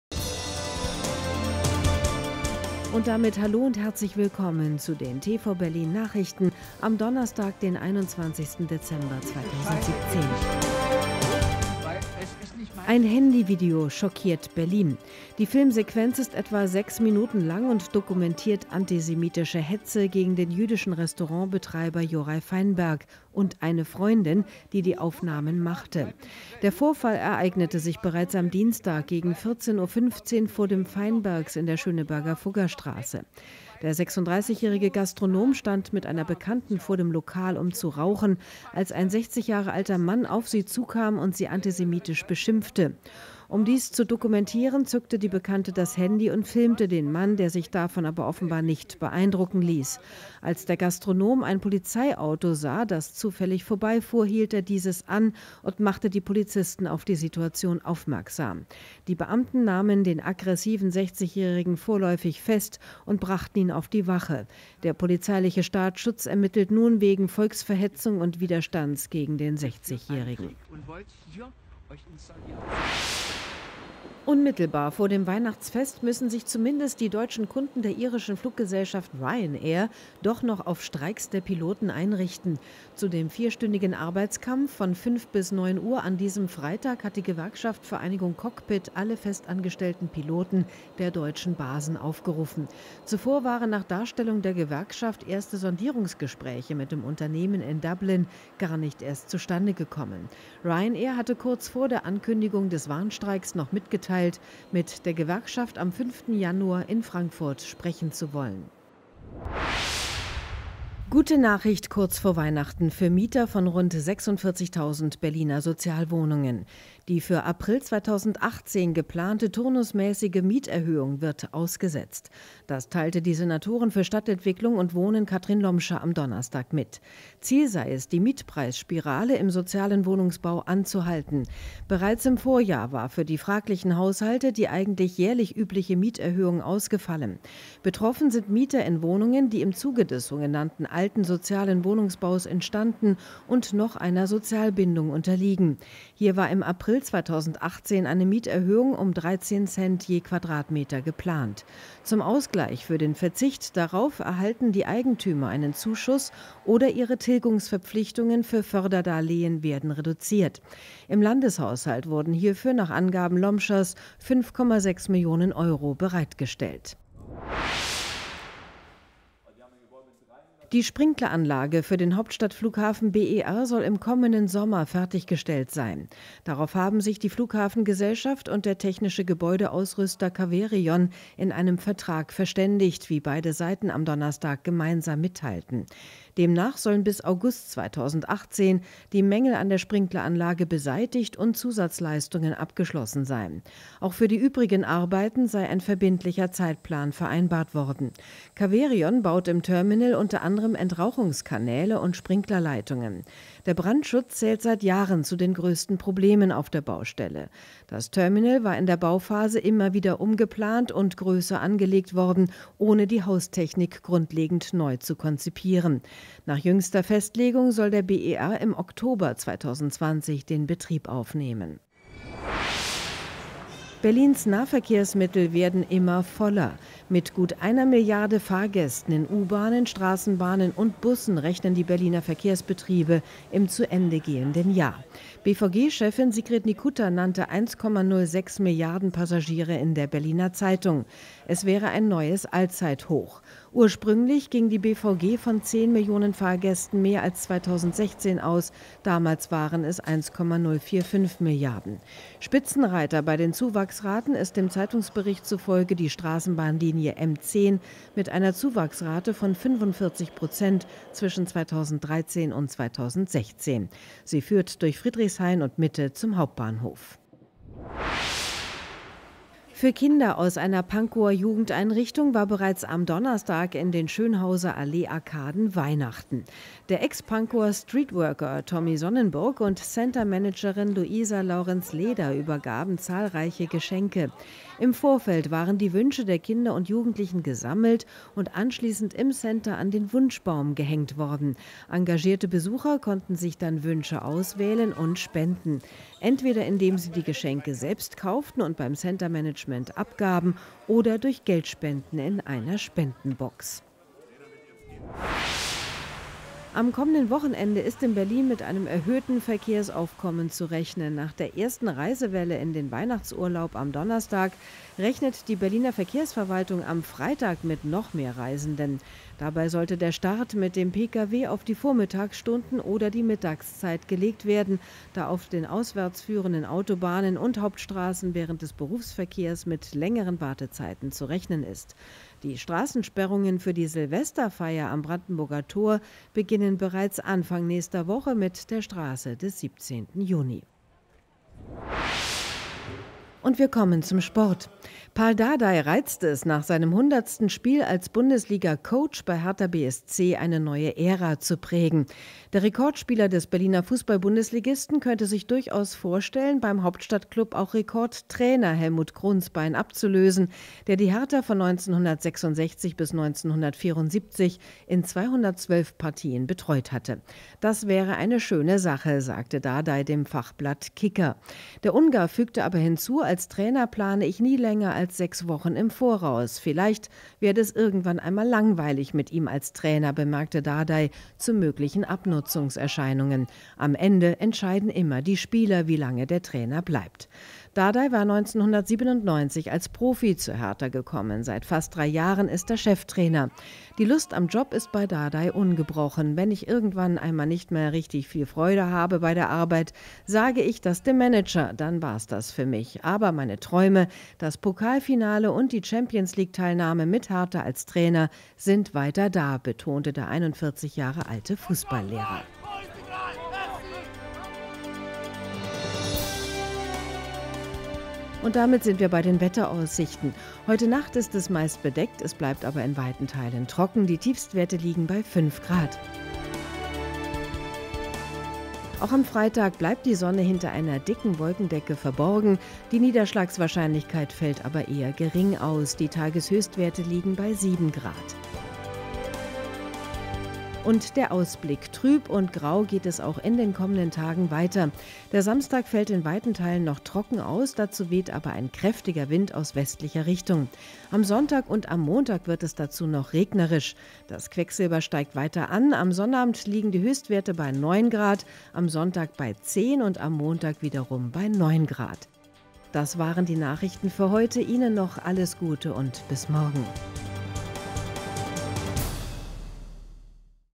tv.berlin Nachrichten vom 21. Dezember 2017
Themen:

- Antisemitische Hetze gegen Restaurant-Betreiber
- Ryanair-Piloten streiken am Freitag
- Mieterhöhungen für Sozialwohnungen ausgesetzt
- Einigung auf Zeitplan mit Gebäudeausrüster am BER
- BVG hat mehr Fahrgäste als je zuvor
- Bescherung für sozial benachteiligte Kinder
- Staugefahr vor Weihnachten
- Dardai-Jubiläum
- das Wetter